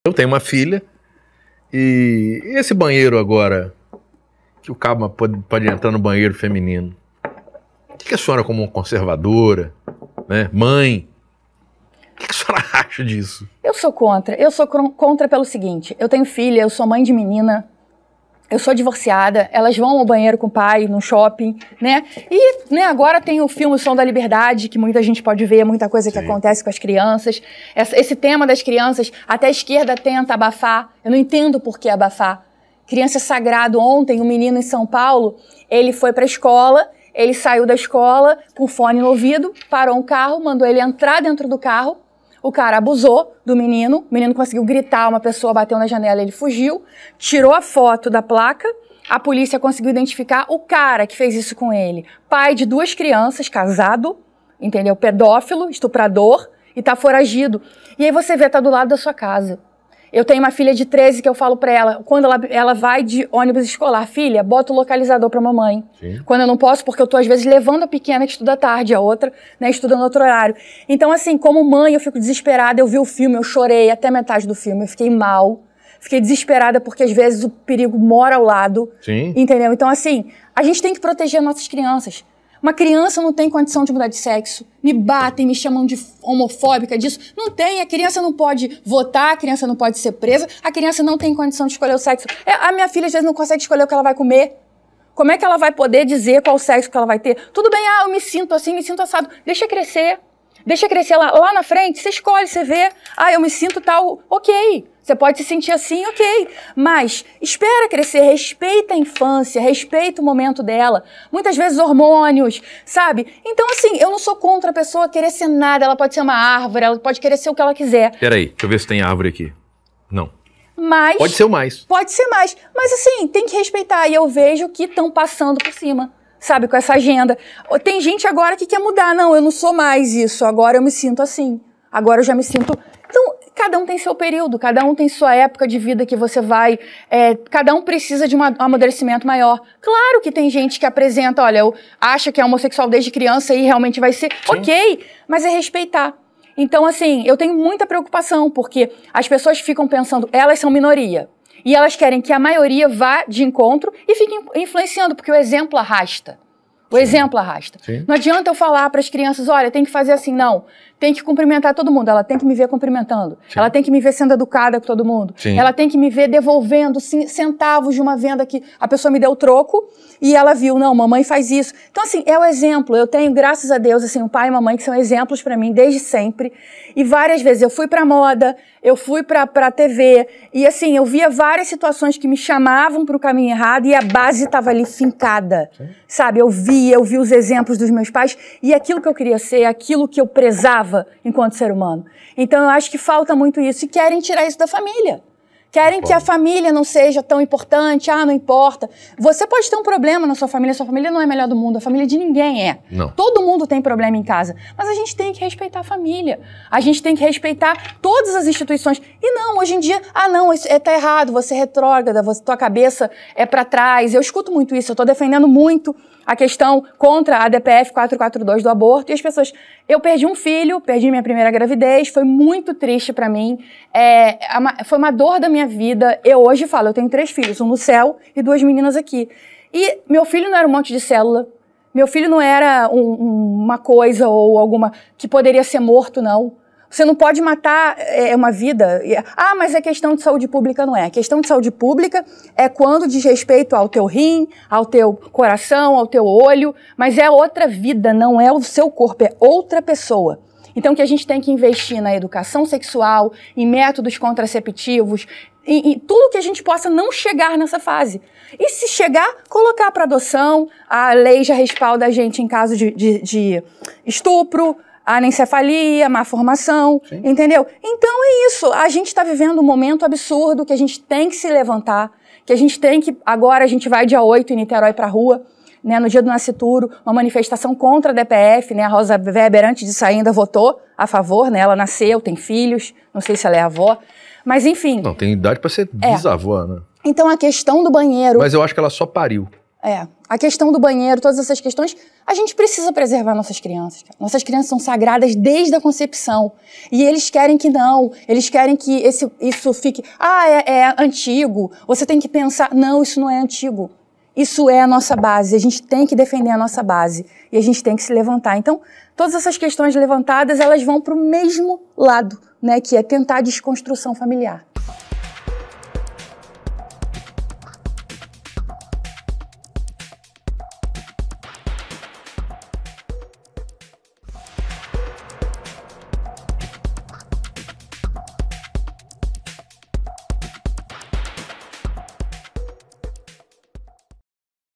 ATENÇÃO! NOSSAS CRIANÇAS ESTÃO NA MIRA DE PERIGOSOS ABUSADORES
Fernanda Louback, Presidente do PL Mulher (Niterói) opina como mãe a respeito dos banheiros unisex que estão sendo implementados no Brasil e lembra de casos citados no filme Som da Liberdade